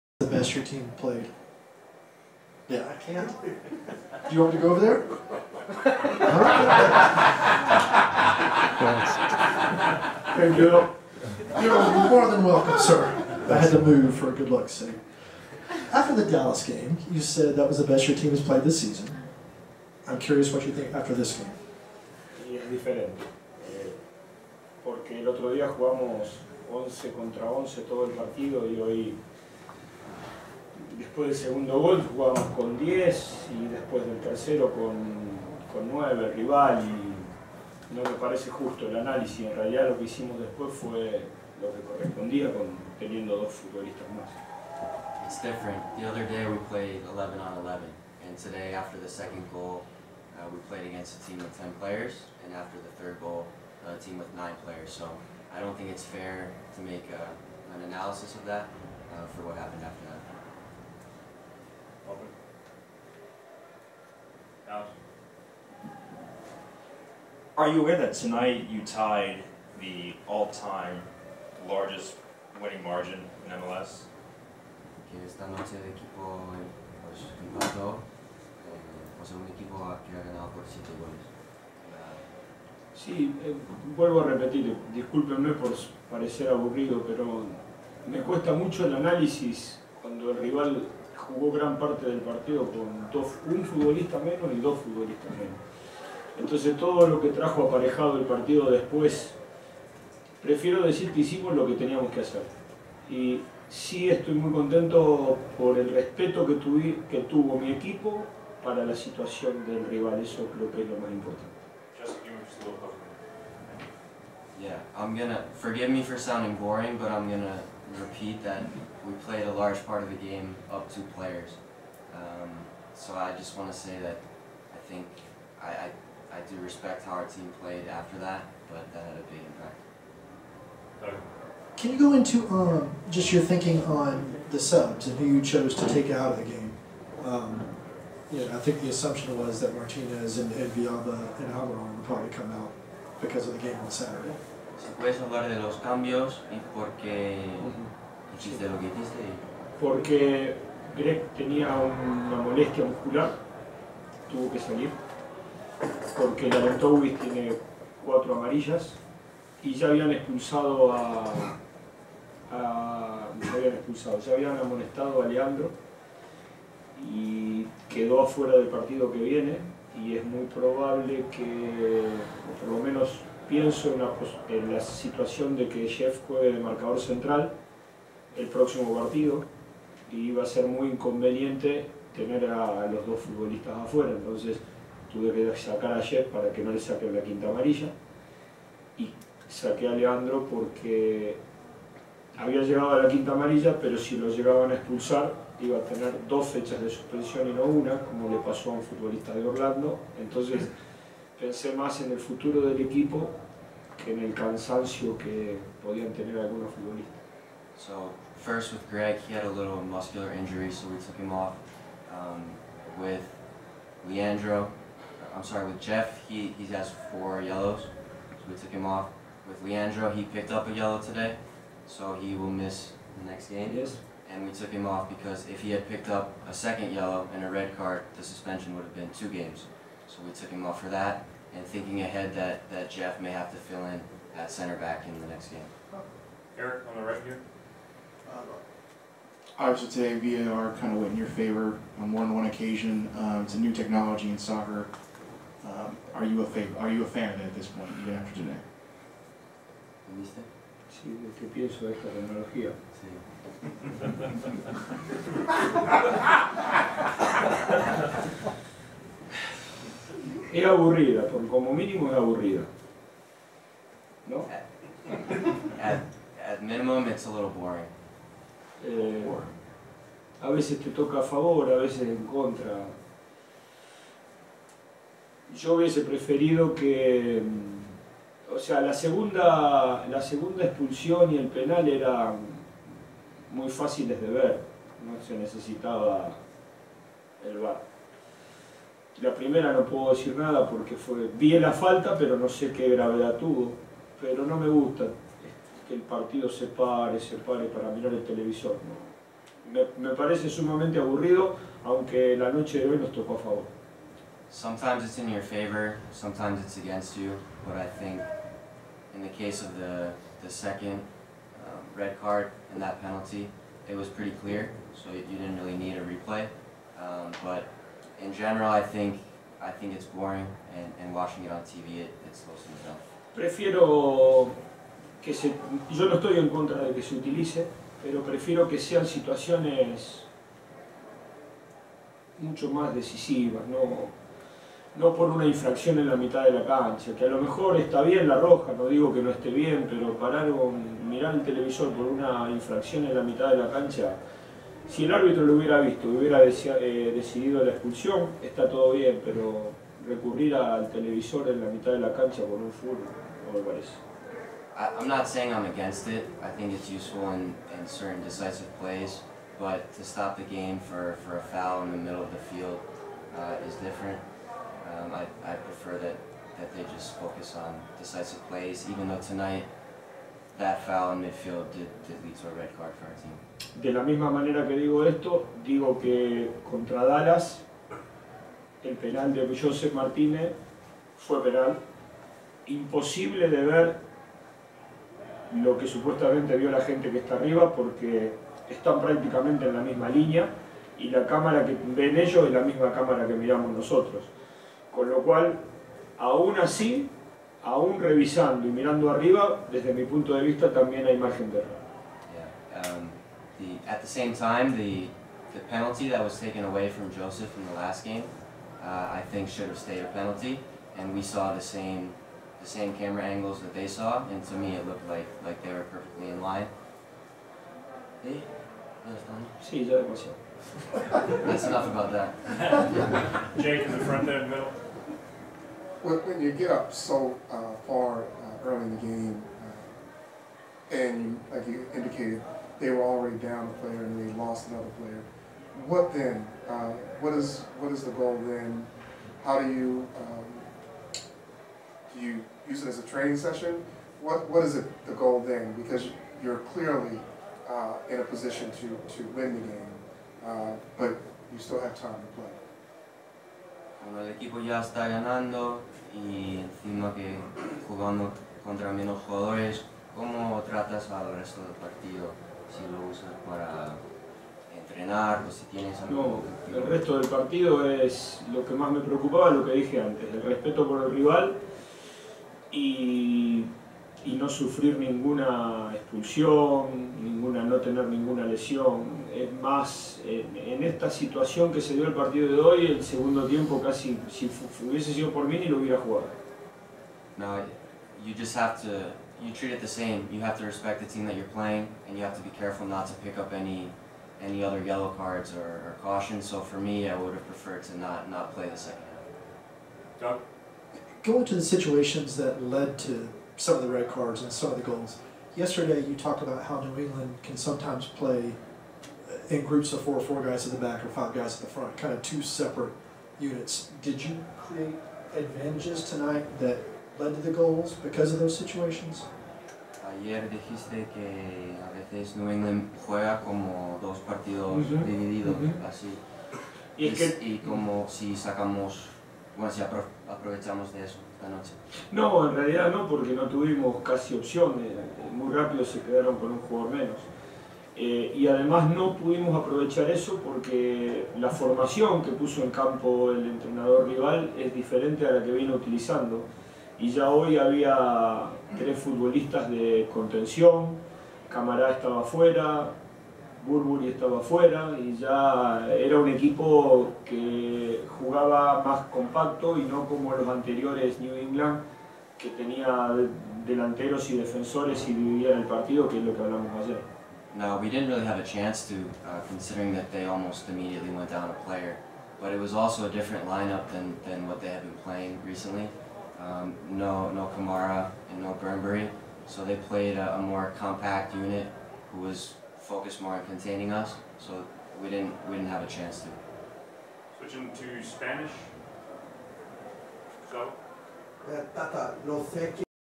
Free Kick: Atlanta United Tata Martino presser 9.13.17
The Sports Inquirer attended the post-game presser of Atlanta United manager Tata Martino following the team's 7-0 home win over the New England Revolution on Sept. 13. Topics included the win, Atlanta's defense not allowing a shot, a Major League Soccer record, playing at home and substitution patterns. Atlanta returns to action on Saturday at home against Orlando City.

El Sports Inquirer asistió al presionador del torneo de Atlanta Tata Martino después de la victoria del equipo por 7-0 sobre el New England Revolution el 13 de septiembre. Los temas incluyeron la victoria, la defensa de Atlanta no permitió un disparo, un récord de Major League Soccer , jugar en casa y patrones de sustitución. Atlanta regresa a la acción el sábado en casa contra Orlando City.